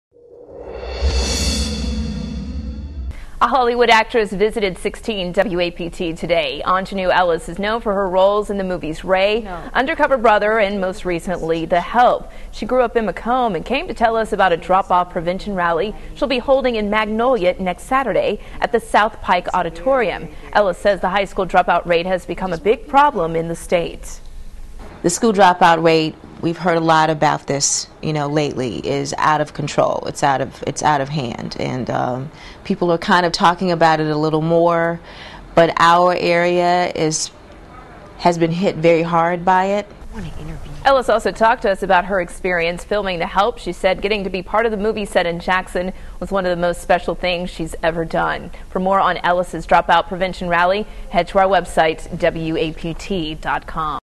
The Help Actress Visits Jackson
Aunjanue Ellis grew up in McComb and came to tell us about a rally she'll be holding in Magnolia next Saturday.